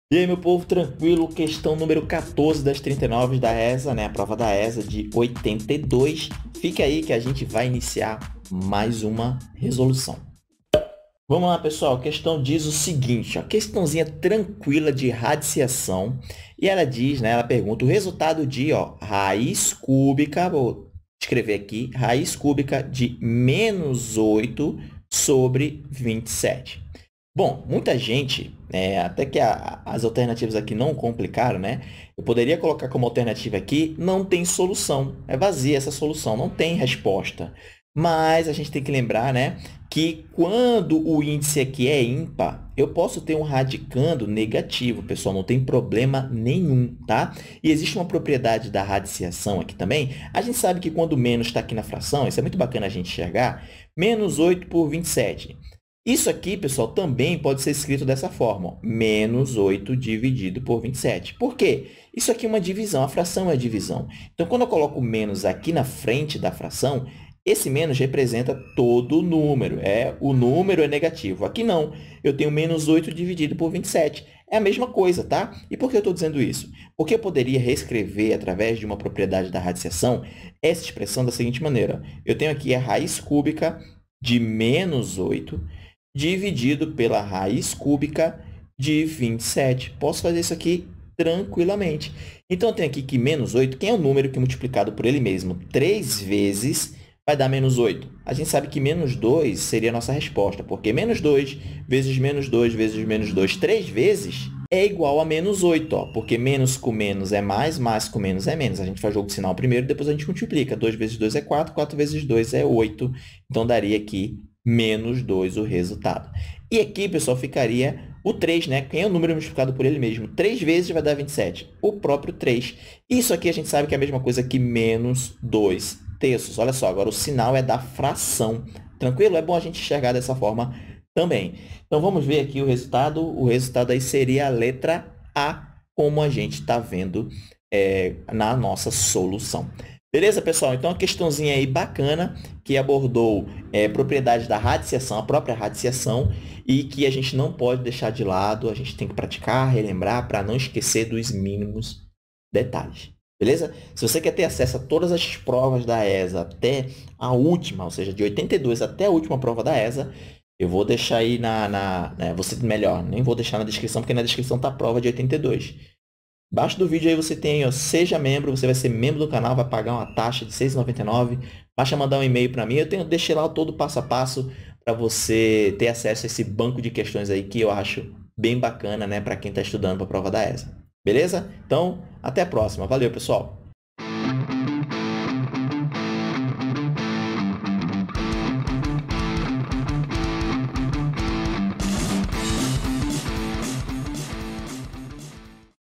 ESA1982 - Q14 - Radiciação - Matemática Básica | Seja Militar
00:00 Inscreva-se e seja feliz
00:11 Solução da Q14

A Inequação do 1º grau é extremamente importante no entendimento do sinal da função e entender sua solução é algo que você, sargento, precisa saber. Construa sua base da melhor maneira possível estudando essas questões, para aprender propriedades novas e ganhar confiança. Não se desespere, pois se eu consegui você também vai, apenas resolva junto comigo essas questões de matemática e se torne um militar. Combinado?